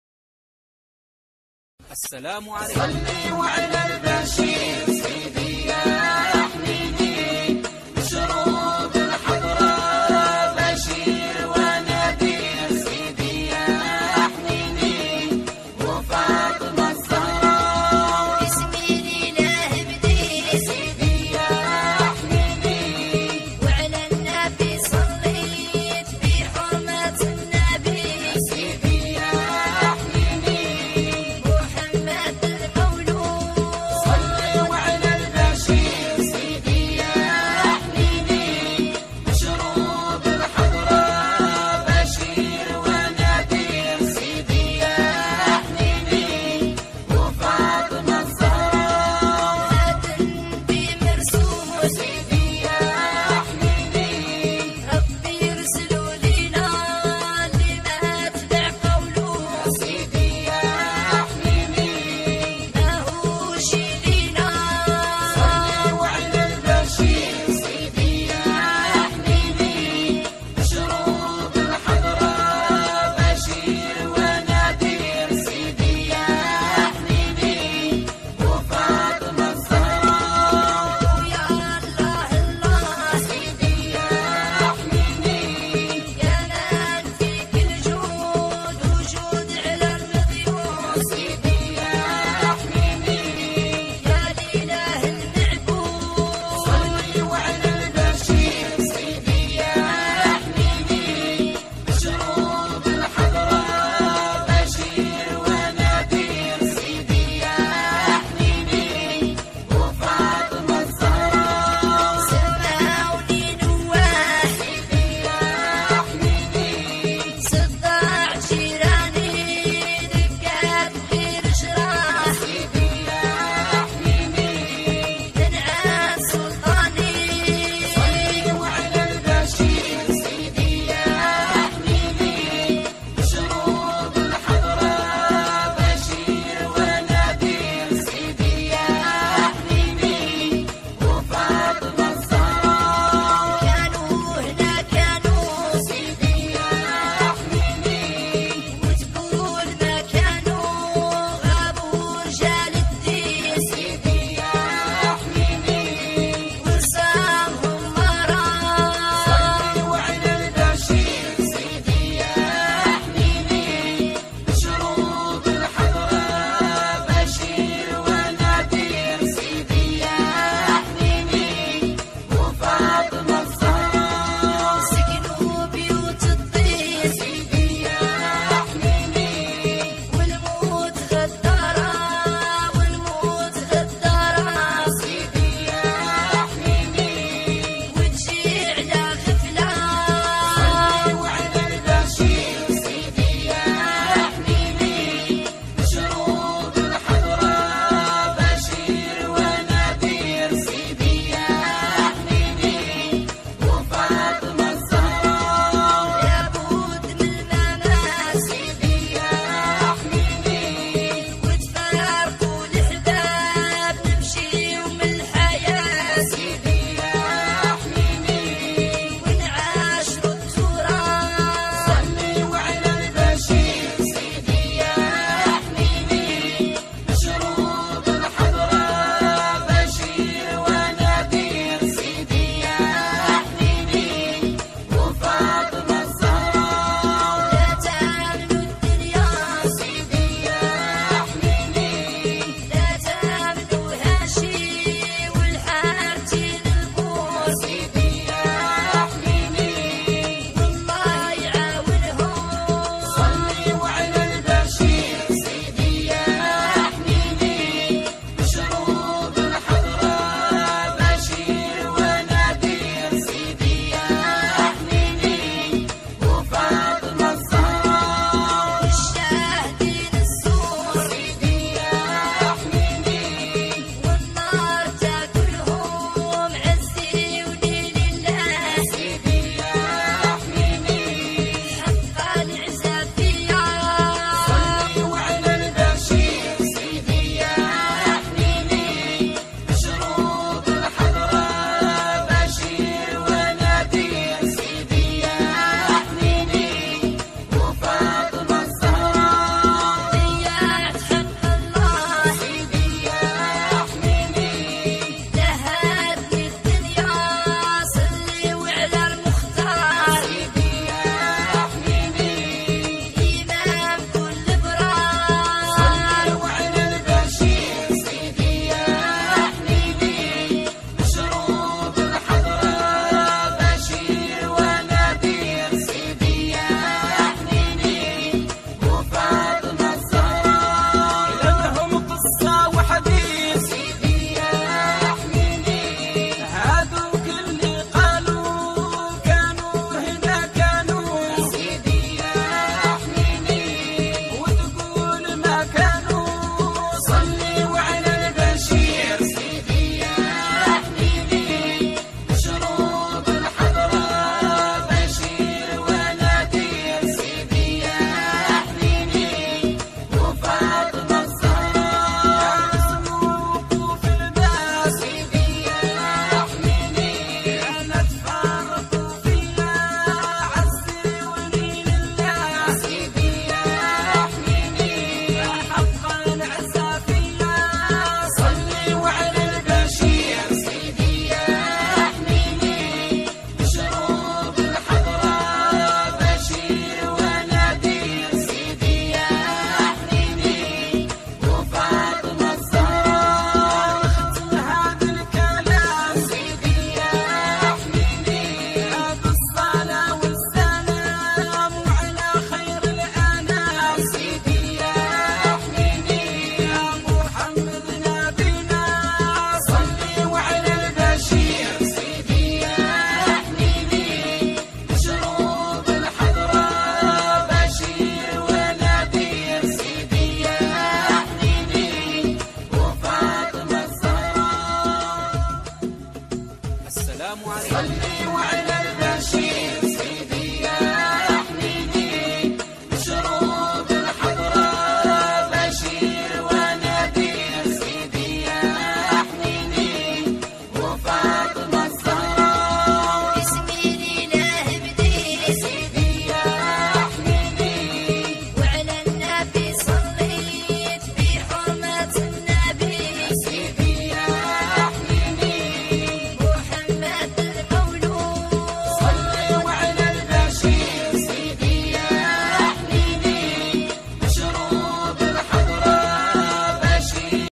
مقتطفات رائعة للغاية من ربوع وطني الحبيب بعد سقوط الثلوج مناظر 🥶بترد الروح 🥰😘
ثلوج#المدية البليدة سطيف عنابة حملة توصيلي# 10الف